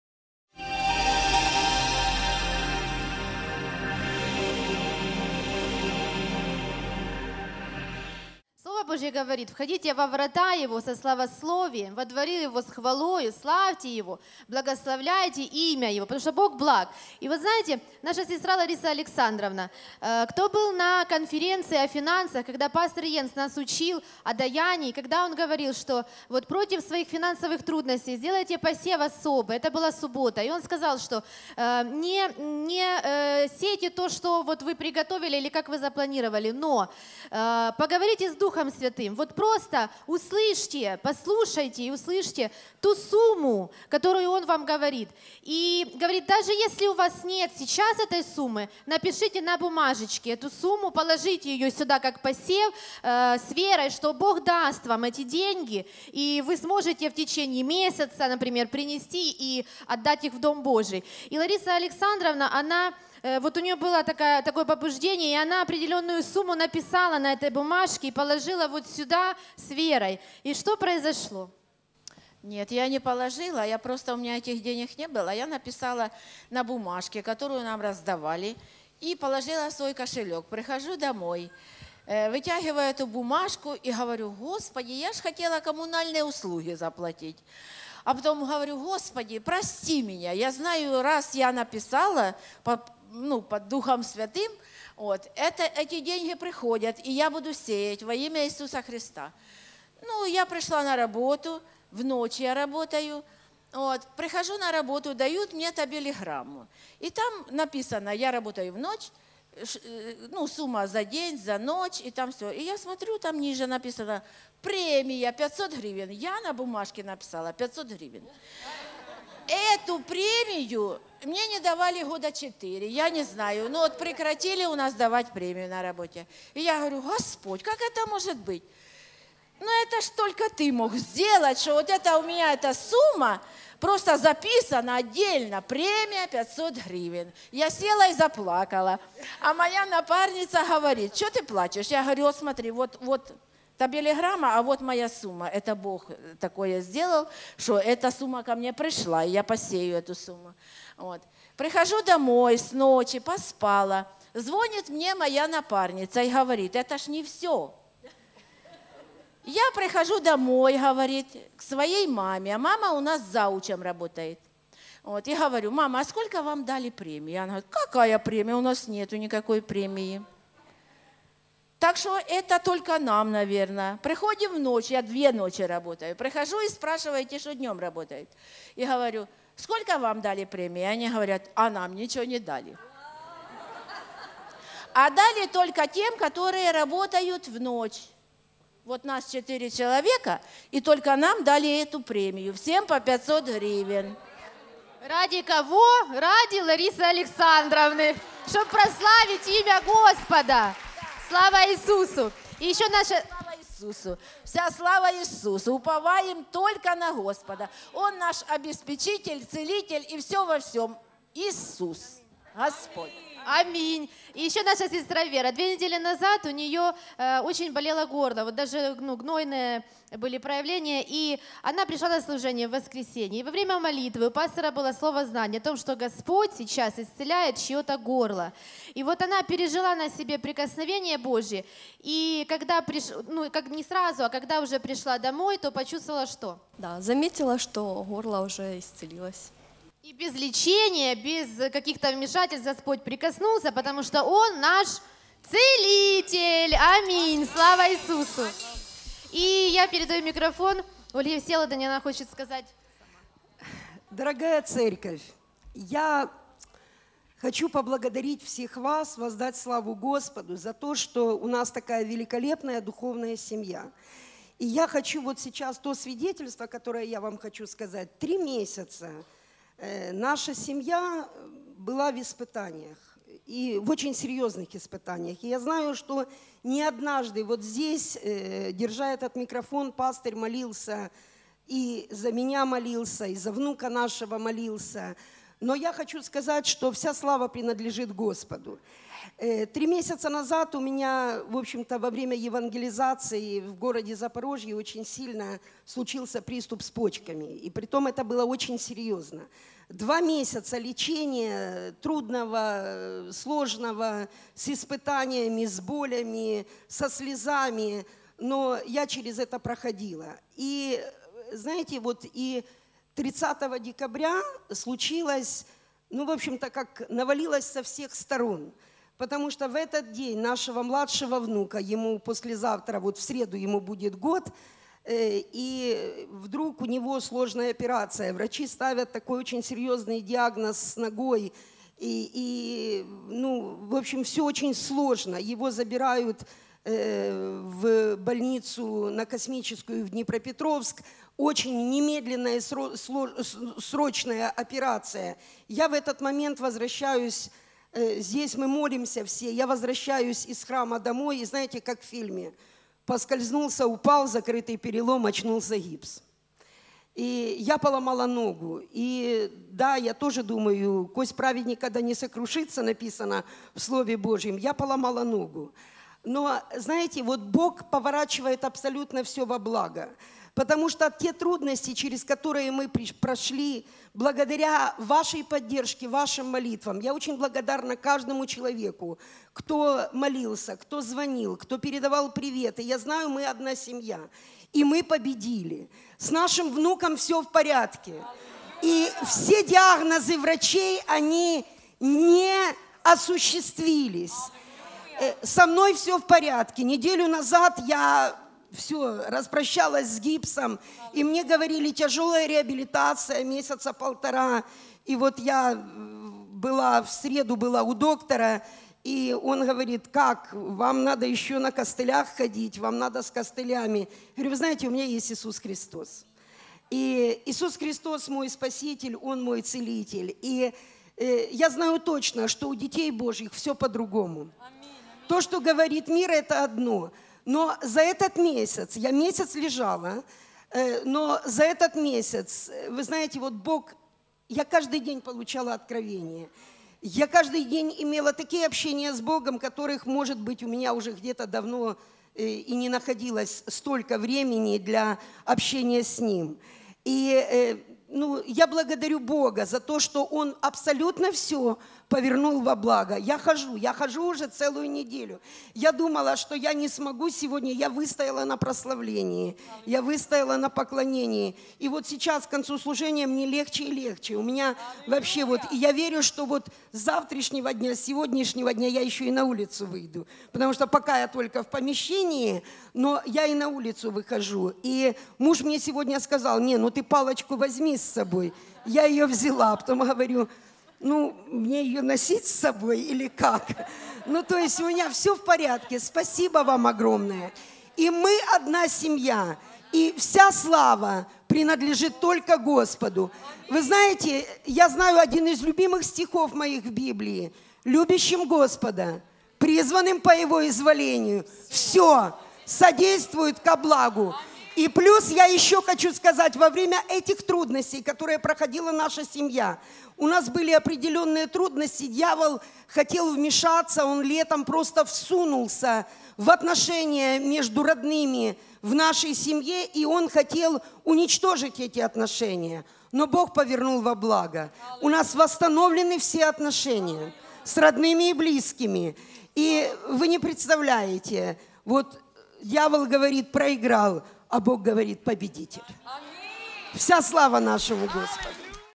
05.02.17 Свидетельства славы Божьей
Старший пастор церкви "Еммануил" Вадим Шипилов (Каменское, Украина)
The senior pastor of the church "Emmanuel" Vadim Shipilov (Kamenskoe, Ukraine)-